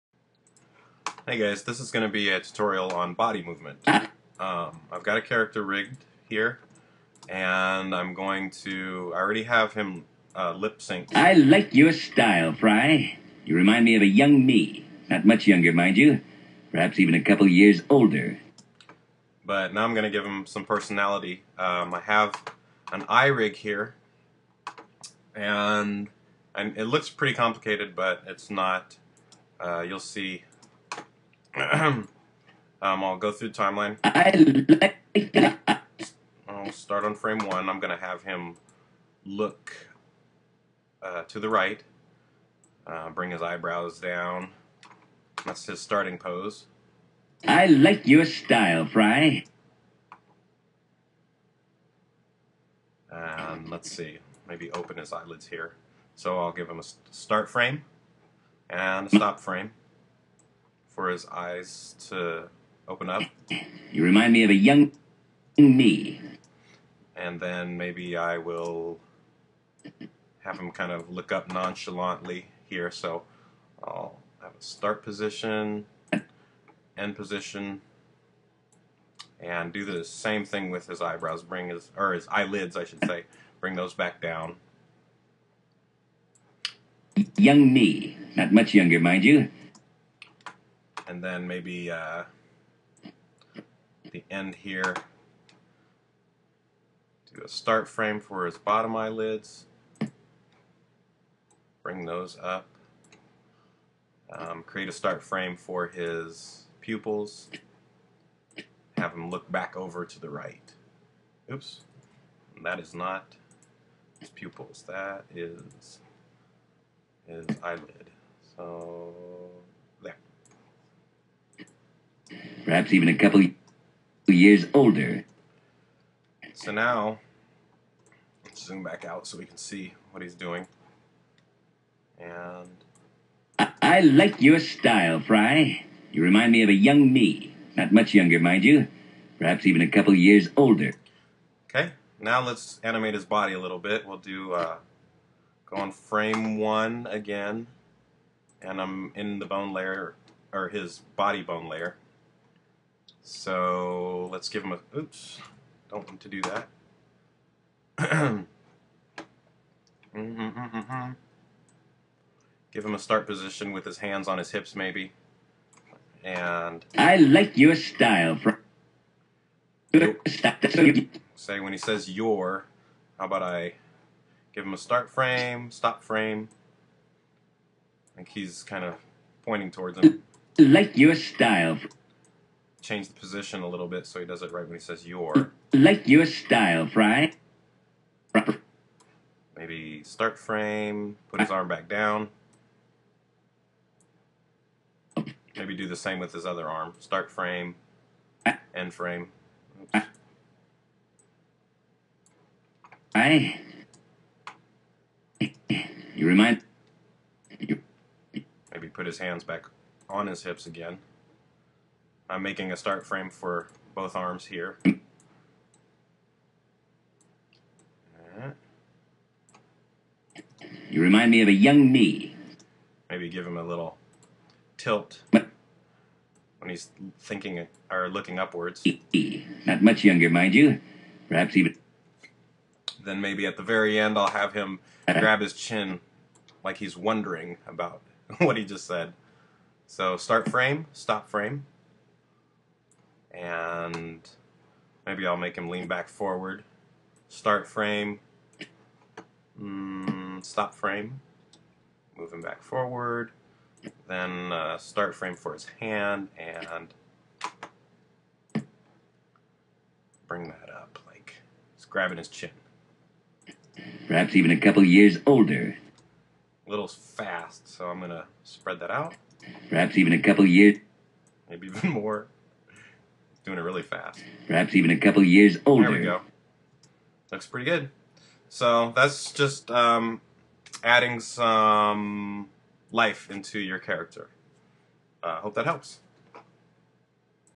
Adding movement to a character.mp4
This is a tutorial for adding movement to a character that is already rigged with bones. It's less a tutorial and more on how to make key frames for quick movements. This is assuming you already know how to rig a character with bones.